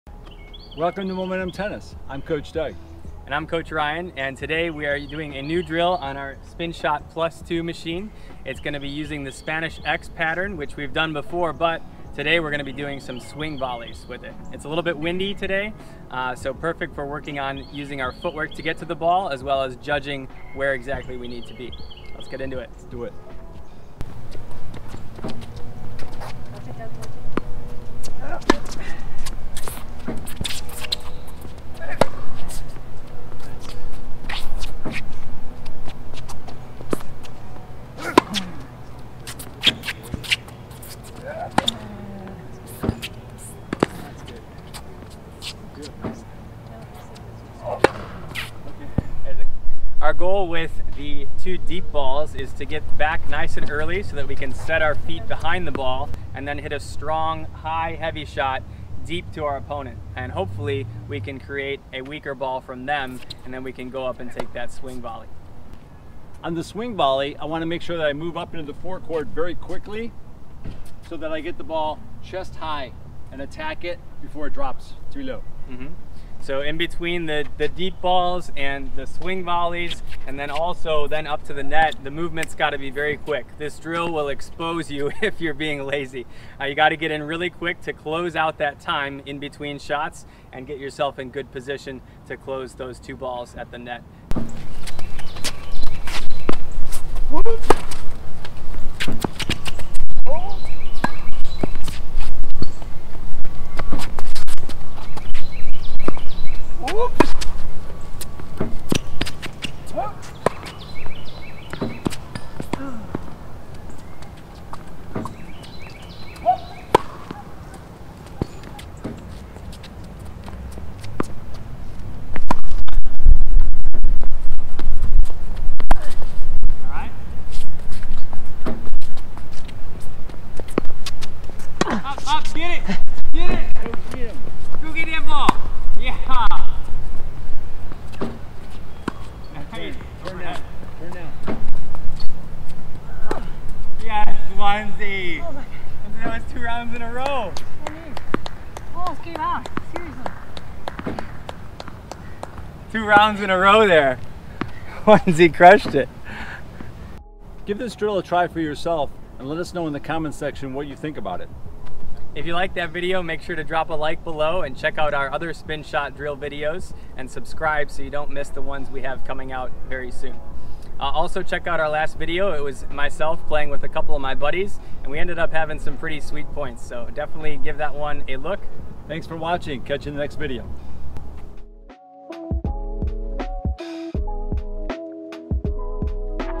SWING VOLLEY Spanish X Drill - Spinshot Tennis Ball Machine (DIFFICULT)
The classic Spanish X Drill with swing volleys to work on aggressive play. This drill is perfect for working on beating "pushers" and "moonballers" and definitely very handy for doubles as well.

Come train with us in beautiful Charleston, SC! 🌴